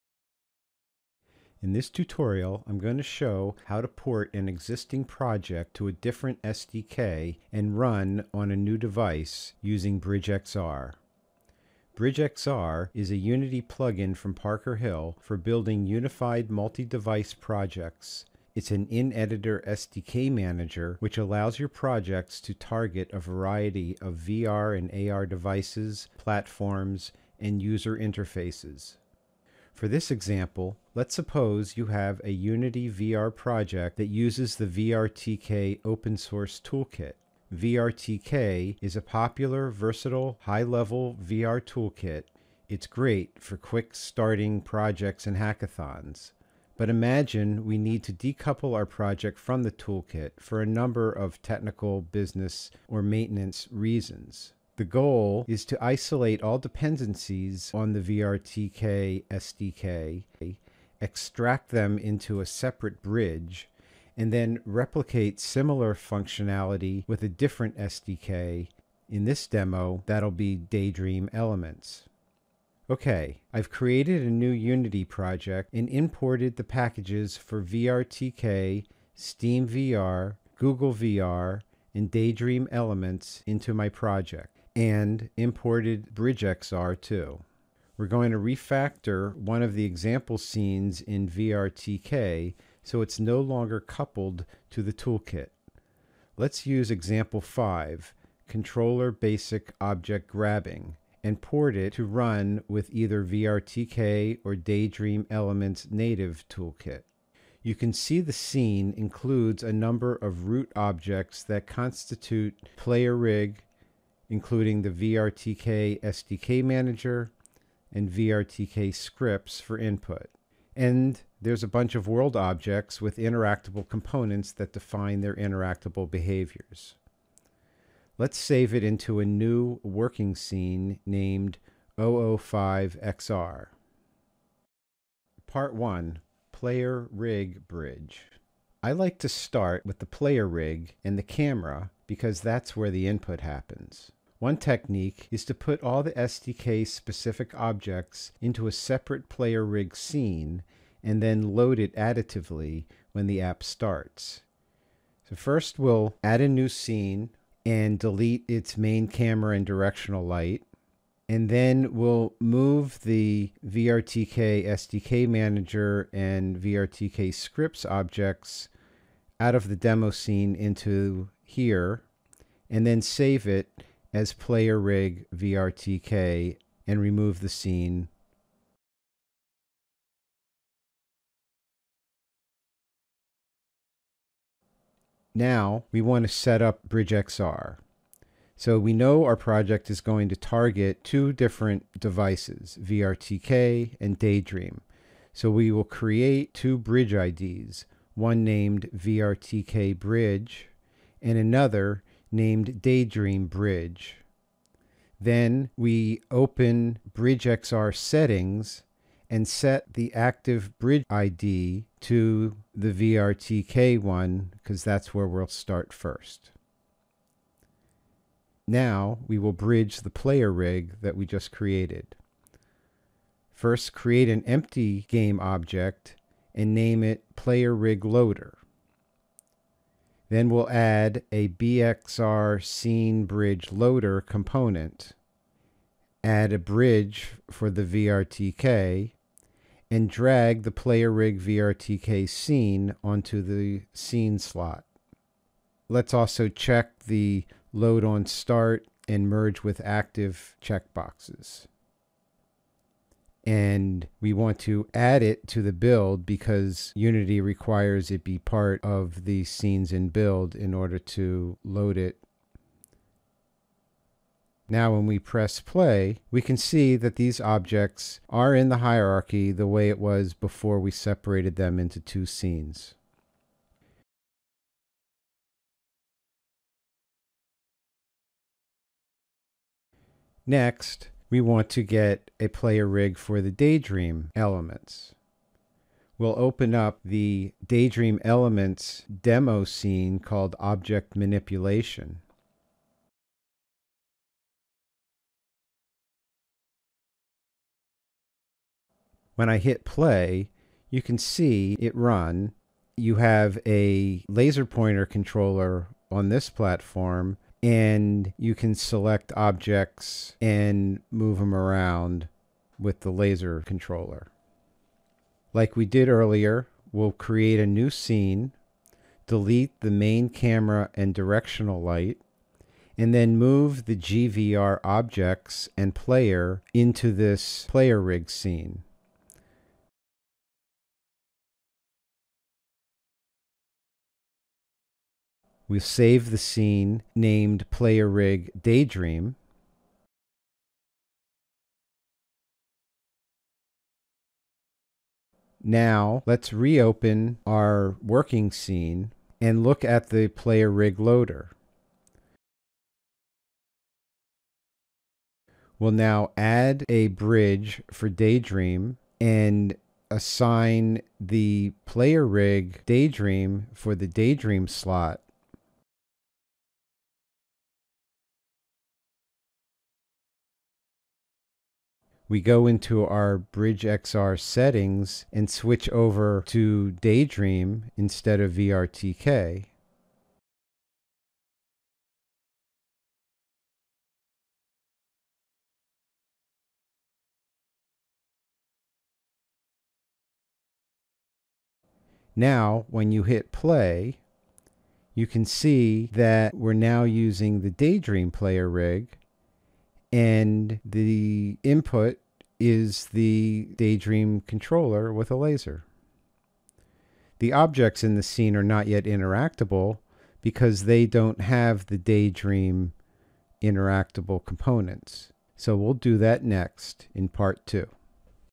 Part 1: BridgeXR vrtk to daydream elements demo
In this video we show how to port an existing Unity project to use a differerent SDK using Parkerhill BridgeXR.

For this example, let’s suppose you have a Unity VR project that uses the VRTK open source toolkit. VRTK is a popular versatile high-level VR toolkit, it’s great for quick starting projects and hackathons.

But Imagine we need to decouple our project from the toolkit, for a number of technical, business, and or maintenance reasons.

The goal is to isolate all dependencies on the VRTK SDK, extract them into a separate bridge. And then replicate similar functionality with a different SDK, in this demo, Daydream Elements.

LINKS:
On the Unity Asset Store